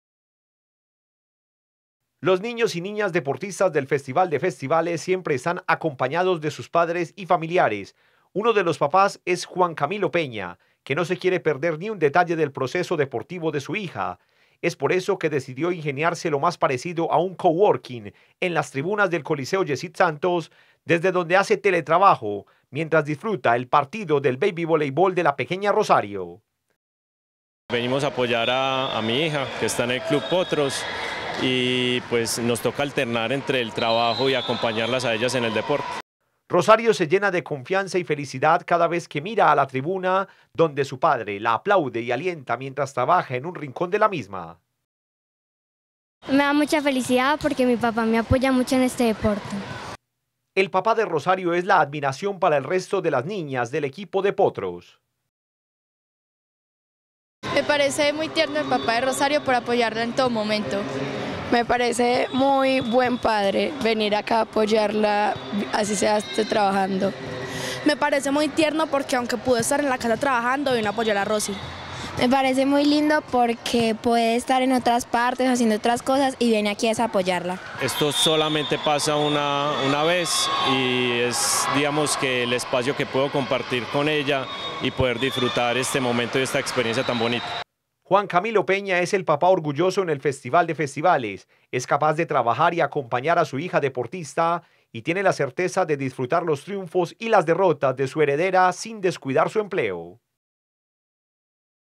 Teletrabajo en el Baby Voleibol [Festival de Festivales] Telemedellín
Entre el trabajo y el deporte 🏐👨🏻‍💻, don Juan Camilo Peña se las ingenió para acompañar a su pequeña Rosario en el BabyVoleibol y poder seguir trabajando, mientras la alentaba desde la tribuna 😍 ¡Tan bonito! 🥰

¡Historias que solo pasan en el FestivalDeFestivales, ElEncuentroDeLosSueños! 🤩